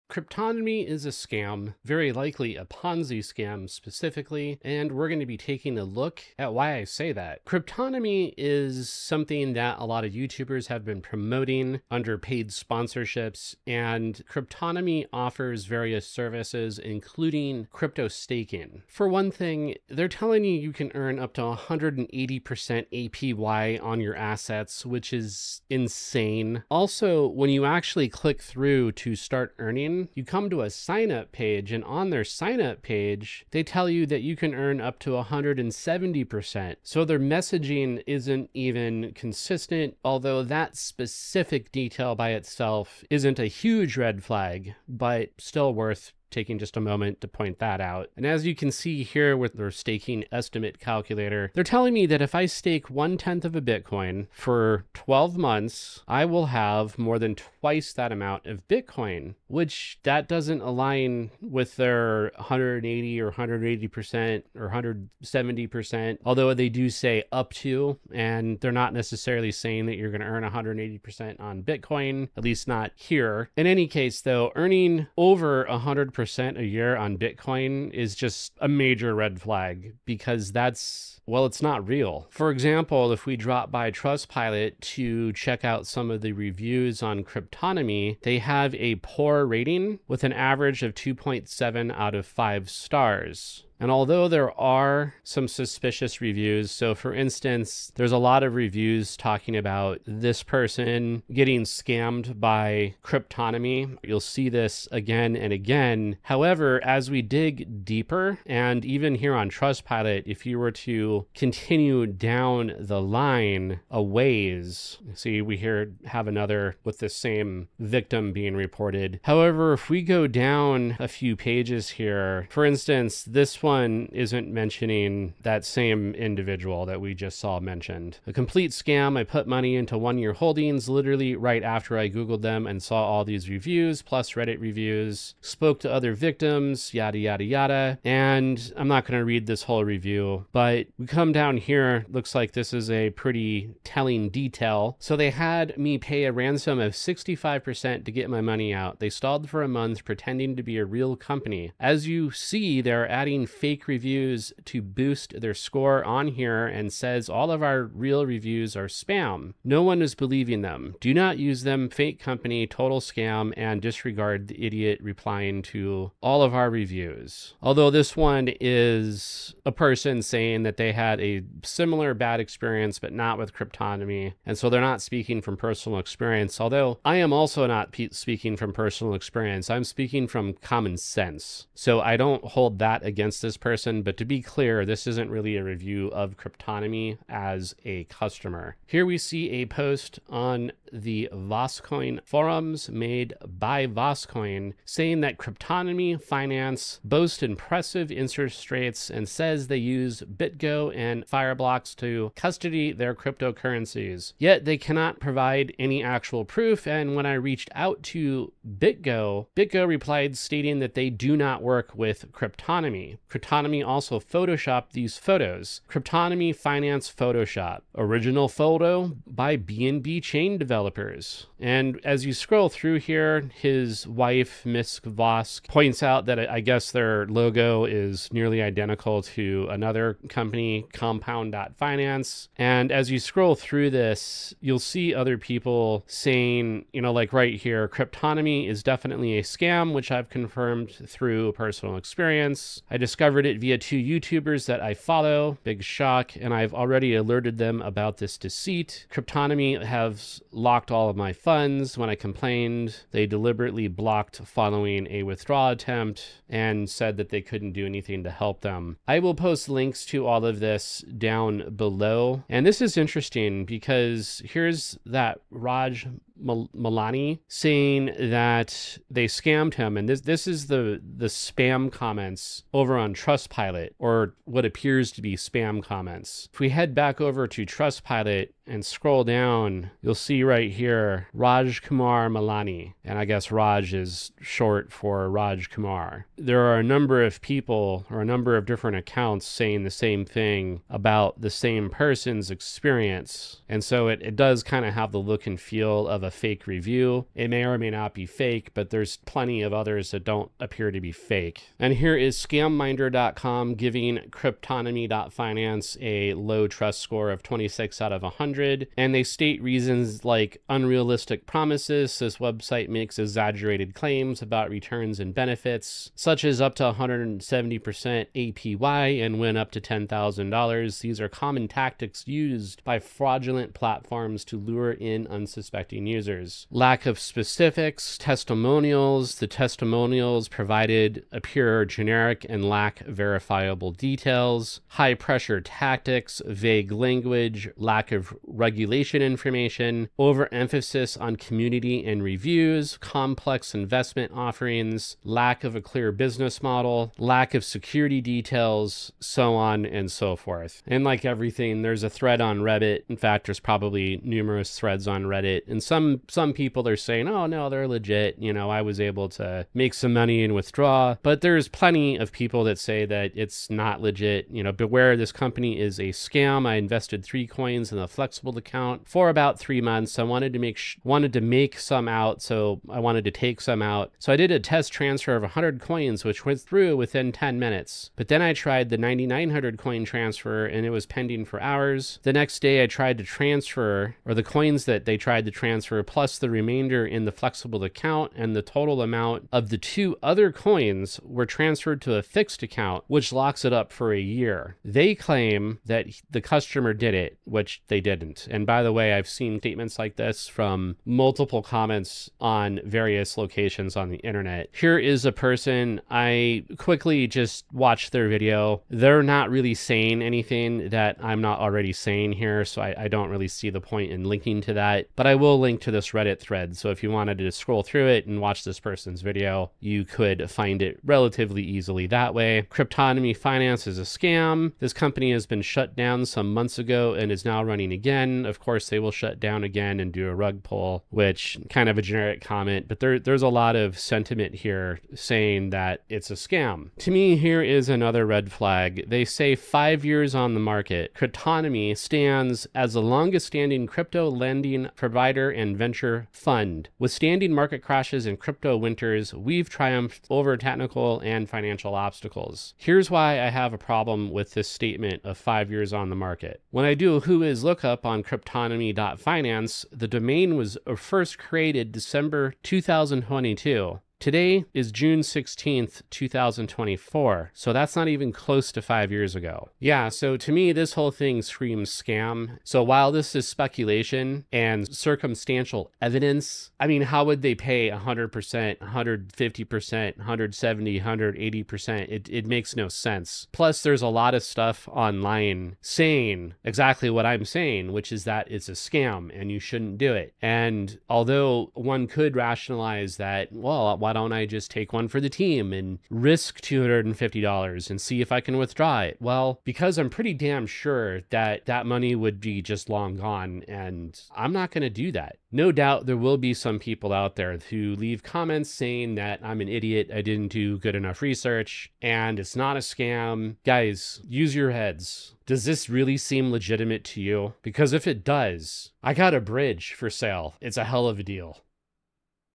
Cryptonomy Is A Scam
Don't get robbed by yet another crypto scammer. Cryptonomy is clearly a scam, likely not the first these people have ran, and unfortunately probably not the last.

GeoIP restricted? Get a VPN!